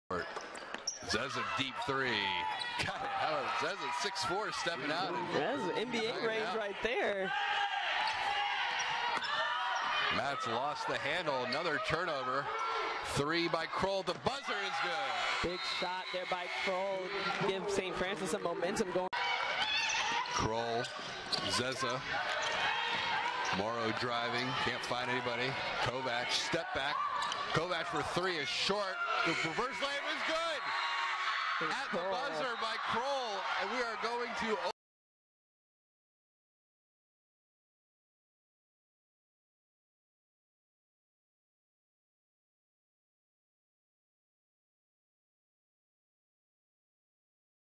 Portland Highlights
A pair of buzzer beaters and the celebration from Saint Francis' 29-point comeback win at Portland 12/20/17